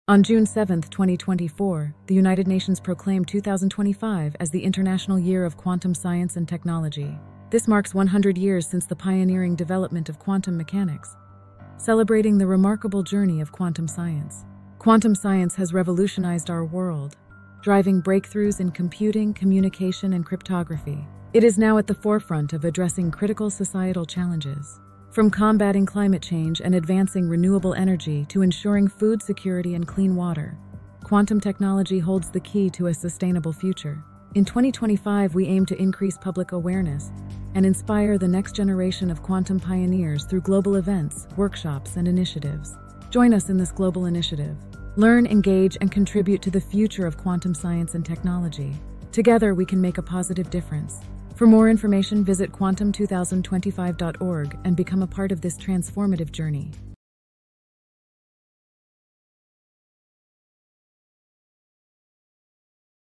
Celebrating Quantum : The International Year of Quantum Science and Technology 2025
Explore the groundbreaking proclamation by the United Nations marking 2025 as the International Year of Quantum Science and Technology. Join us on a journey through the history, significance, and future impact of quantum science. Discover how quantum technology is revolutionizing computing, communication, and addressing global challenges such as climate change and energy sustainability. Let's celebrate quantum's role in shaping our future and inspiring the next generation of innovators.

For more information and to get involved, visit quantum2025.org.